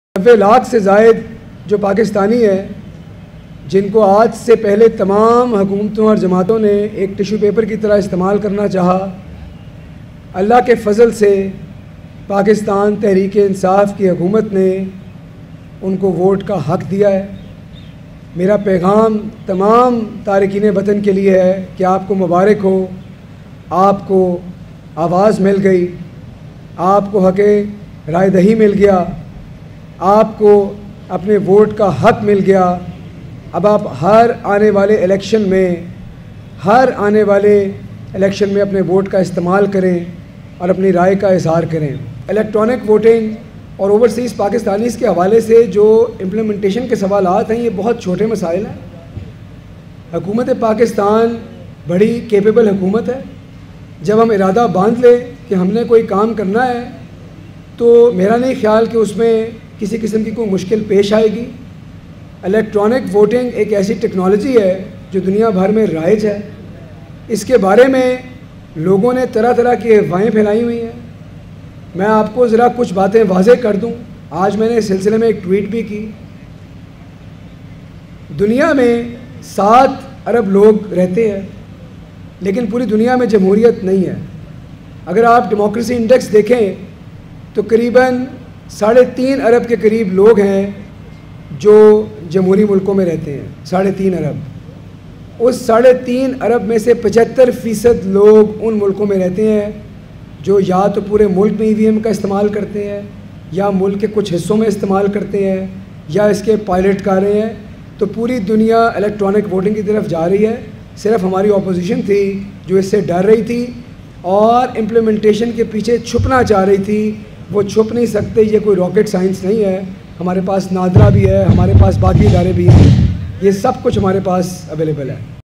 Electronic Voting Machines Give Opposition Electric Shock: Hasaan Khawar
LAHORE: Special Assistant to Chief Minister Punjab on Information and Special Initiatives Hasaan Khawar on Thursday said the electronic voting machines had given the opposition an electric shock.
“The EVMs are going to end match-fixing forever,” Hasaan Khawar said in a tweet alleging that the use of technology made the Pakistan Muslim League-N and Pakistan Peoples Party nervous for they had always relied on manipulation and match-fixing.
The Punjab government spokesperson said 75 per cent of the democratic countries in the world either used the EVMs nationwide or in parts of the country or had EVM pilots underway.
“Use of electronic voting machines: There are 7 billion people in the world. 3.4 billion live in democracies (Democracy Index). 75% of these (2.6+ billion) live in countries that either use EVMs nationwide or in parts of the country or have EVM pilots underway,” he wrote.
At the joint session of the parliament on November 17, the government backed by its allies passed amendments to the Elections Act, 2017, which allows usage of the EVMs and gives right to overseas Pakistanis to vote in the general elections.
Following passage of the bills, the opposition staged a walkout in protest saying that the bills were passed with less number of votes than required.
Hasaan Khawar said the Sharif brothers did politics from abroad but did not give the right to vote to overseas Pakistani citizens.
“Those who have their own properties abroad and their children are also overseas, how could they oppose vote of the overseas?” he wondered.